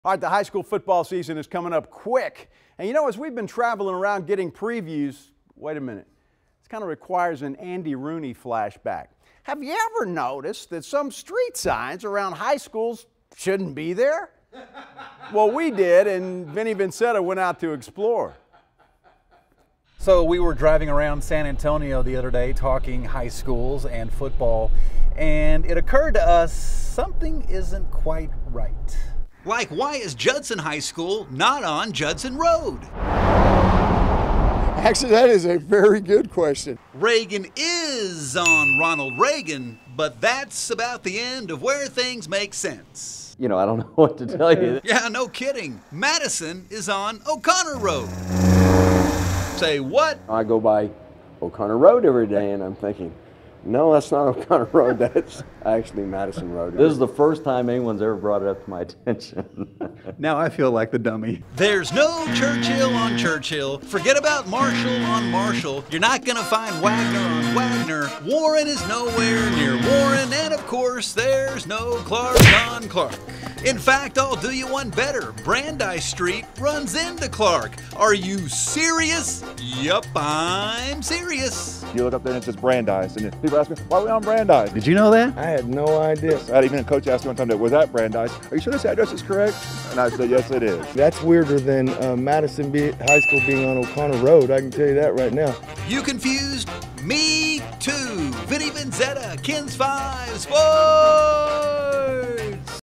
SA high school street signs don't make sense
Have you ever noticed that some street signs around high schools shouldn't be there?  We did, and Vinnie Vinzetta went out to check on it.